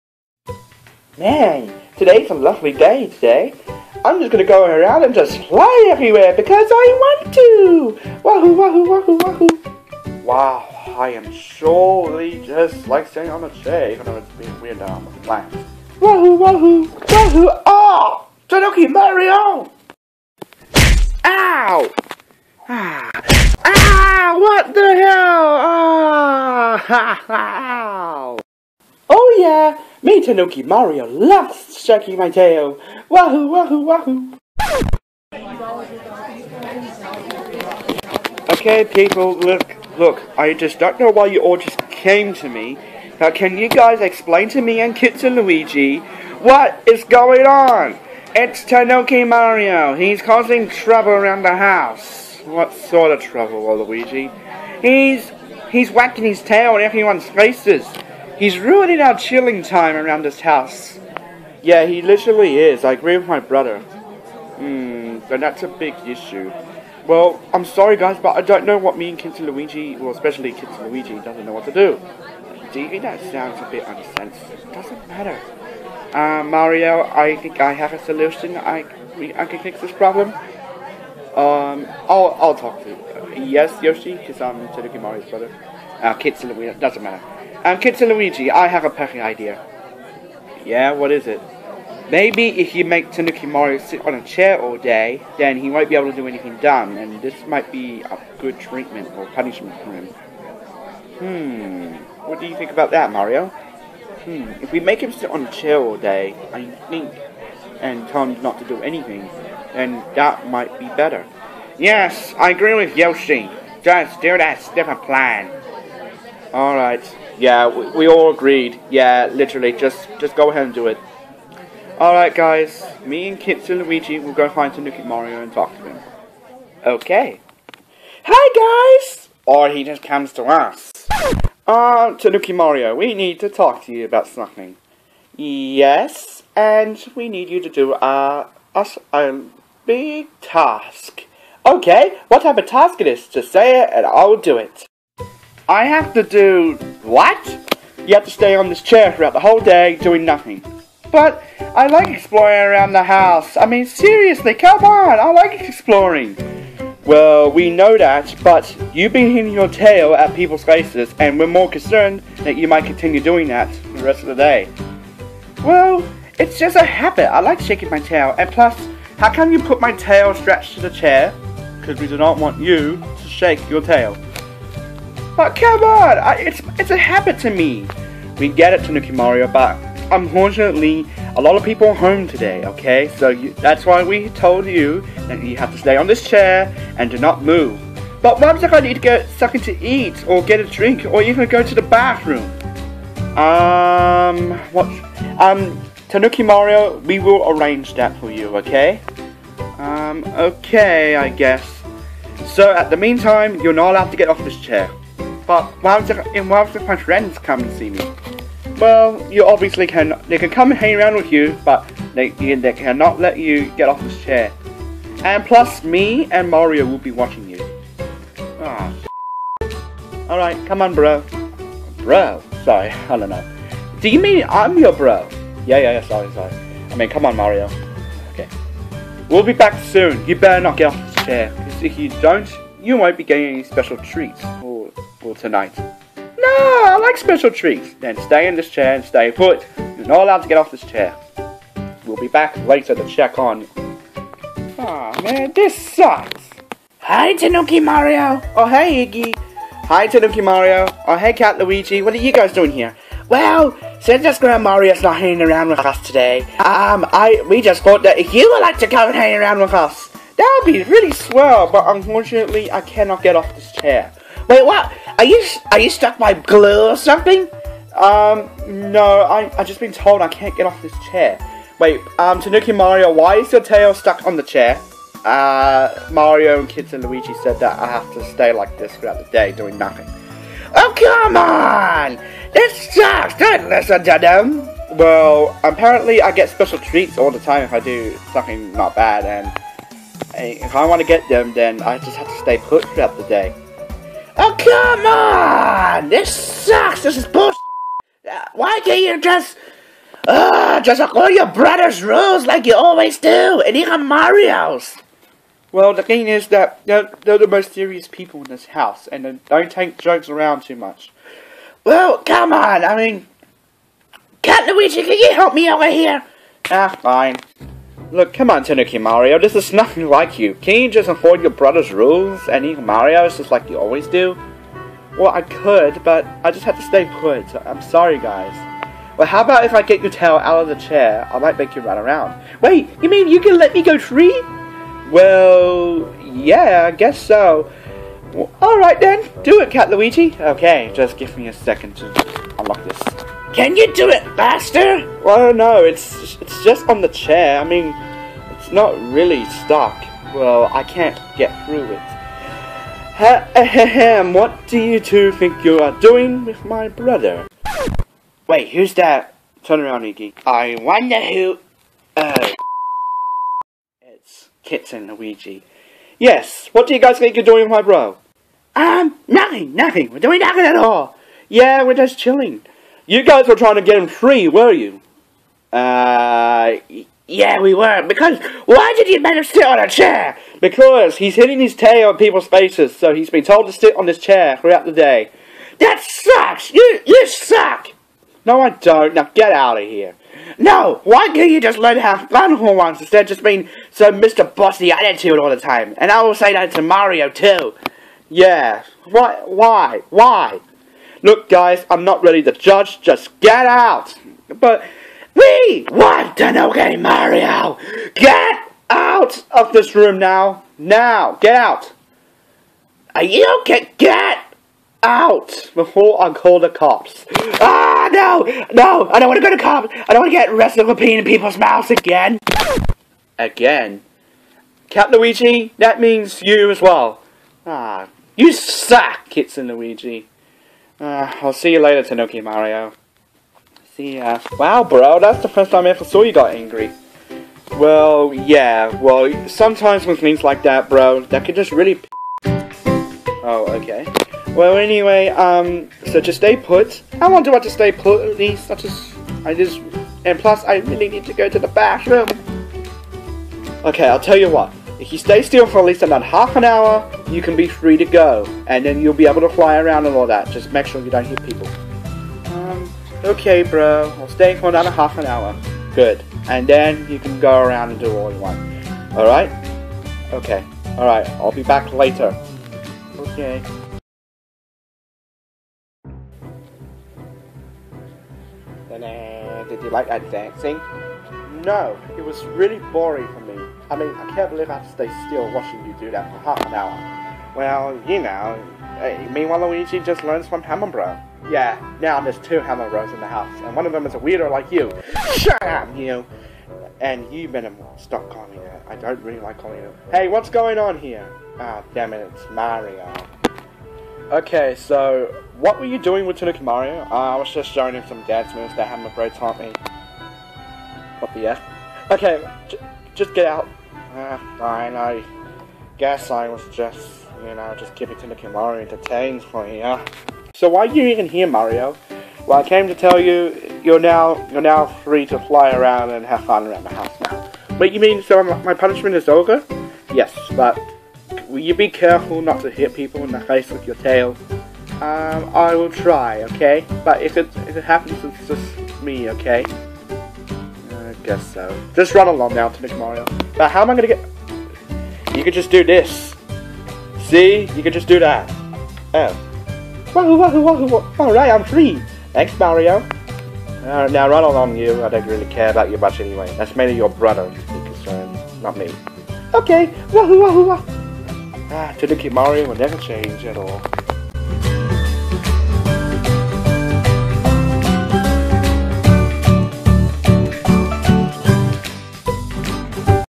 BMF100 Movie: Tanooki Mario's Tail Problem!
Tanooki Mario has been hitting people in the face with his tail, so Mario & Kitsune Luigi decided to make him sit on a chair all day & do nothing.